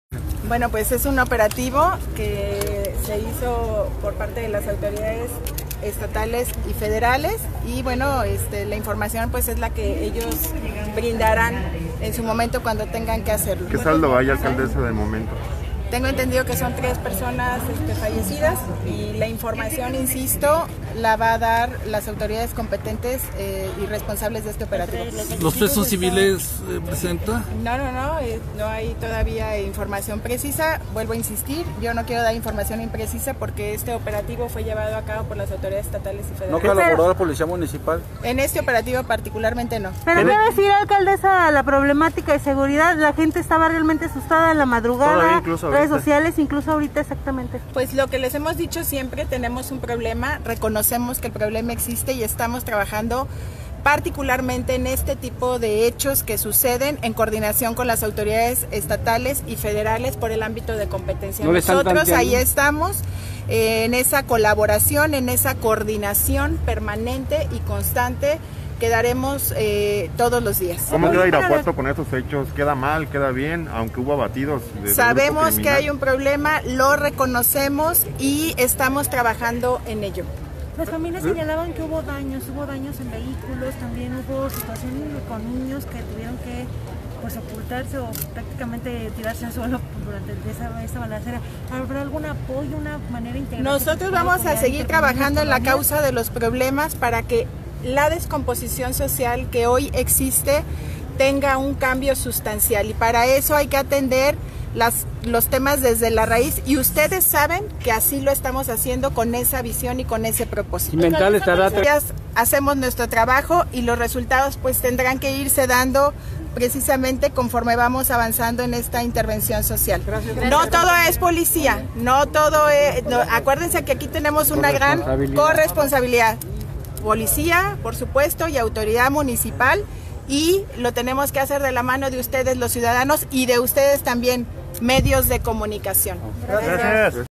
Tras enfrentamiento en Irapuato, alcaldesa urge a atender las causas de raíz
Irapuato, Gto. (Opinión Bajío).- Tras el enfrentamiento entre fuerzas estatales y delincuentes que dejó saldo de 3 muertos, la alcaldesa de Irapuato reconoció que hay un problema de inseguridad y señaló que para reducir esta situación, deben solucionarse de raíz los factores que generan dicha situación